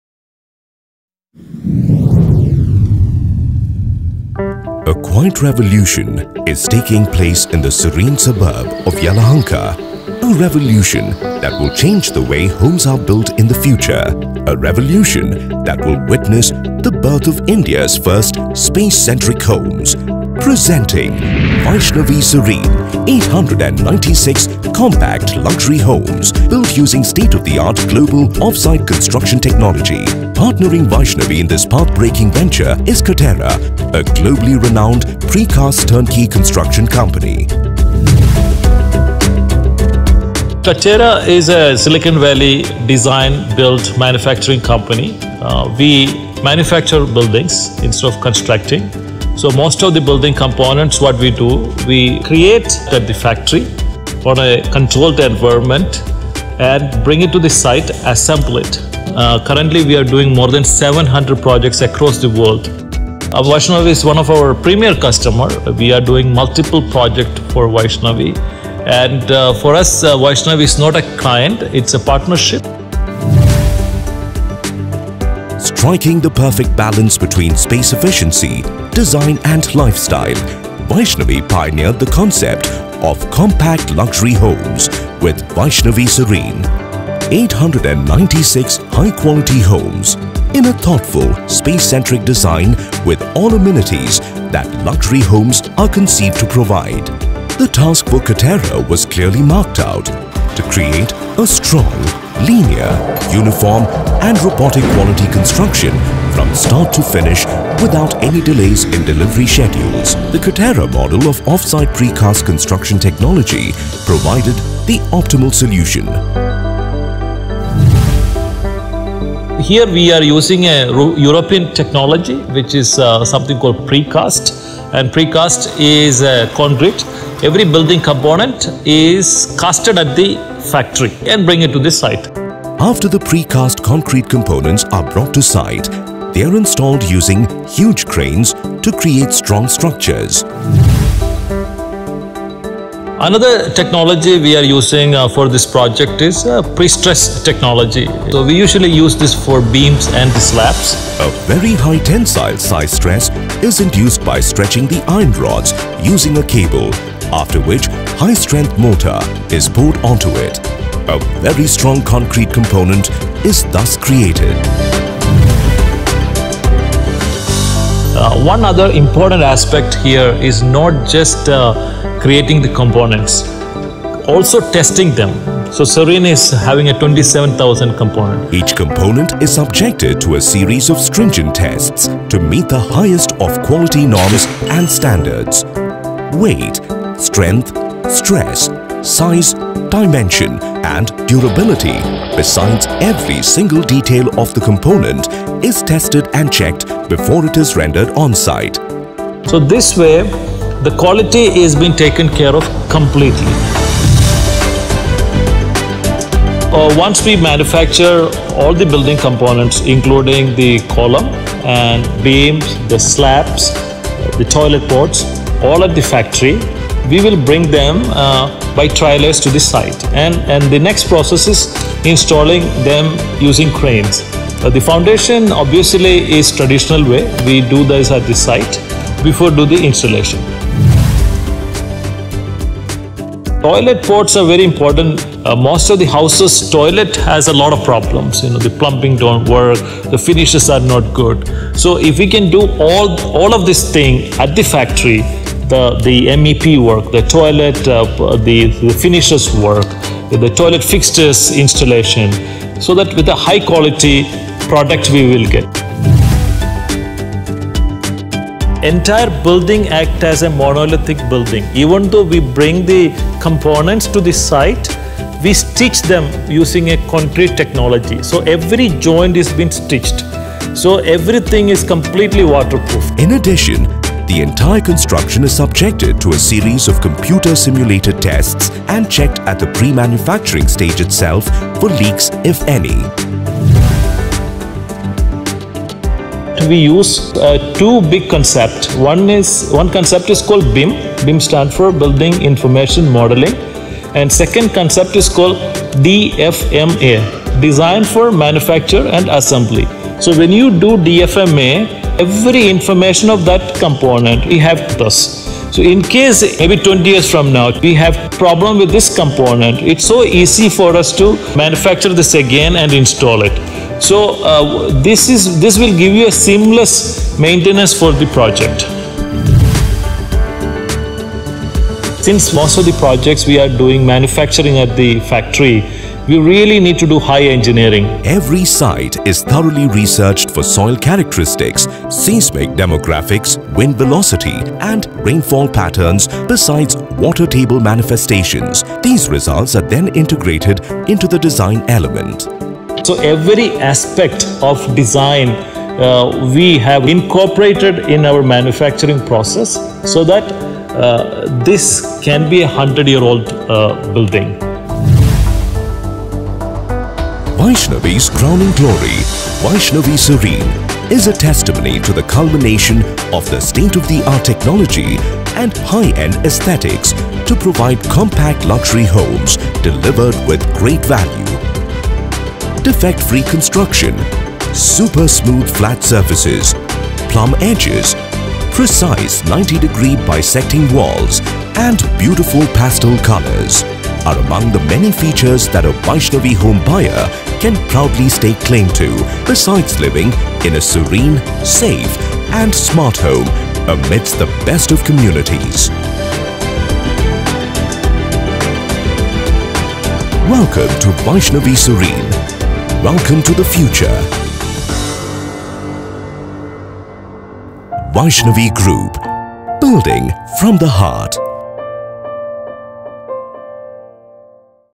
Katerra | Vaishnavi Serene
Katerra is collaborating with the Vaishnavi Group to bring a technology-driven design-build approach to India's housing construction. The 896 unit apartment complex, Vaishnavi Serene in Bangalore, will be manufactured offsite at Katerra's factory.